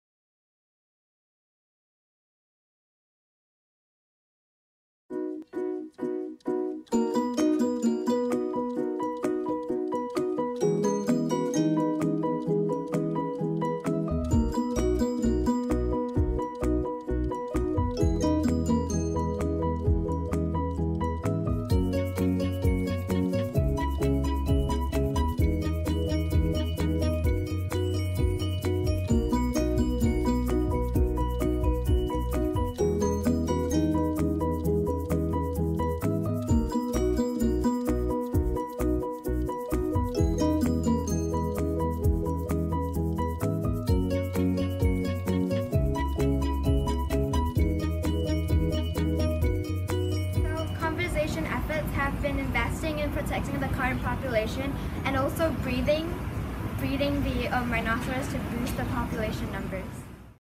Sumatran rhinoceros - 1st - EBC Programme Documentary Competition - Greenview Sec
The Exxon Mobil Endangered Biodiversity and Conservation Programme aims to increase public biodiversity and conservation awareness of Singapore and Southeast Asian biodiversity.

These videos showcase the winners of the documentary making competition in the Secondary School category.

First place: Greenview Secondary School, Group 4, featuring the Sumatran rhinoceros. The team members are: Lim Zhi Xin, Lookhanumanjao Chatchaya, Wayne Song Wan You and Li Wanjun Alyssa.

Second place: NUS High School of Mathematics and Science, Group 8, featuring the Sunda pangolin. The team members are: Soundarajan Keerthana, Zen Hoi and Jin XunZe.

Third place: Evergreen Secondary School, Group 5, featuring the Sunda pangolin. The team members are: Chua Jia Hao Collister, Lee Guo Rui, Lawrence Caisip and Tan Wei Xiang.

We hope that these videos will encourage everyone to learn more about SEA conservation and its issues.